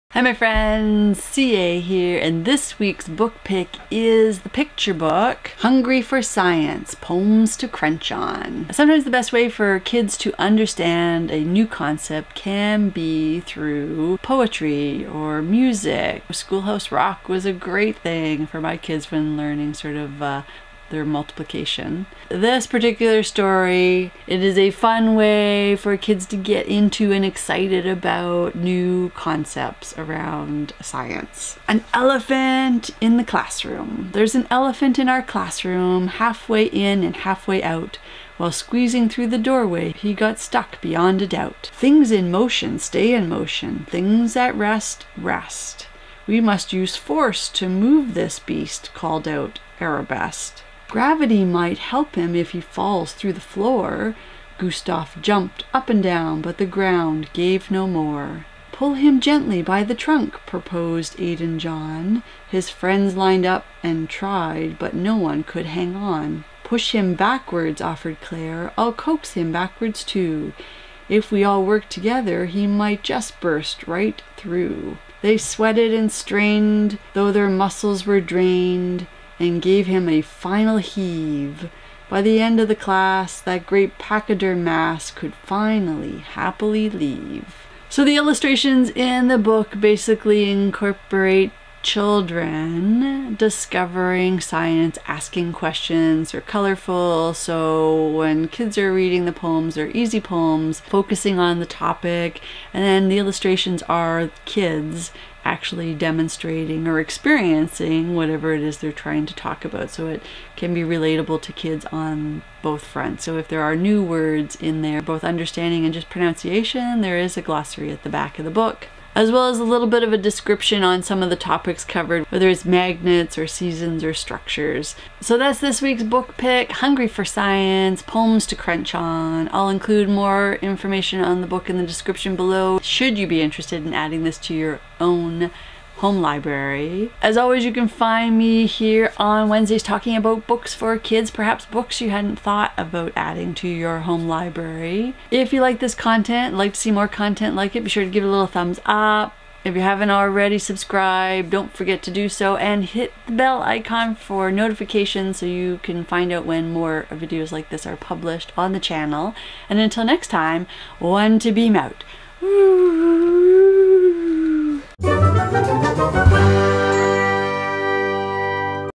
Hungry for Science Poems to Crunch On: Geek Books for Kids
This week's book pick, Hungry for Science: Poems to Crunch On, introduces kids to science in fun rhyme.

Hungry for Science: Poems to Crunch On
Fitzhenry & Whiteside
By Kari-Lynn Winters & Lori Sherritt-Fleming, illustrated by Peggy Collins
32 pages
age 5-8

Check back Wednesday for new recommendations on geek-themed books to add to your child's bookshelf.